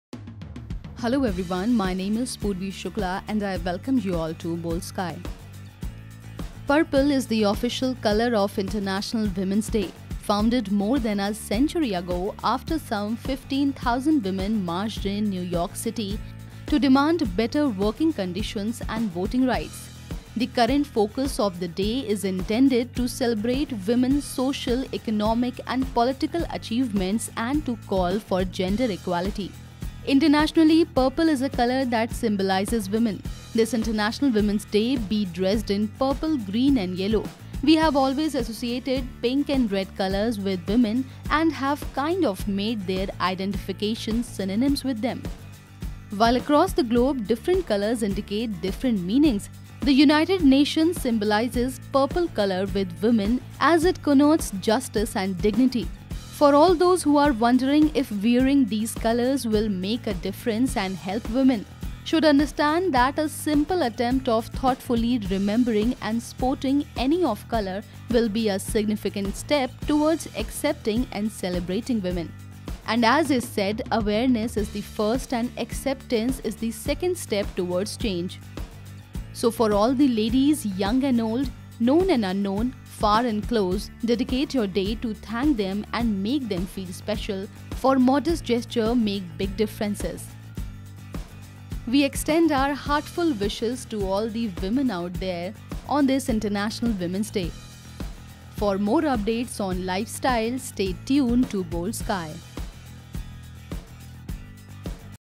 Best Colors to wear on Women's Day: Find out why you should wear Purple | Boldsky
Purple is the official color of International Women’s Day, founded more than a century ago after some 15,000 women marched in New York City to demand better working conditions and voting rights. Internationally, purple is a color that symbolizes women.

इंटरनेशनल विमेंस डे महिलाओ का दिन है, जो की पुरे विश्व में महिलाओं को सम्मान देने, सामाजिक से लेकर राजनितिक जीवन में महिलाओ द्वारा प्राप्त की गयी उपलब्धियों को मनाने और लिंग समानता पर बल देने के लिए मनाया जाता है.  इस दिन उनके द्वारा किये गए कामो की सराहना की जाती है और उनके लिए प्यार जताया जाता है. आईये जानते है की इस दिन पर्पल कलर पहनना क्यों चाहिए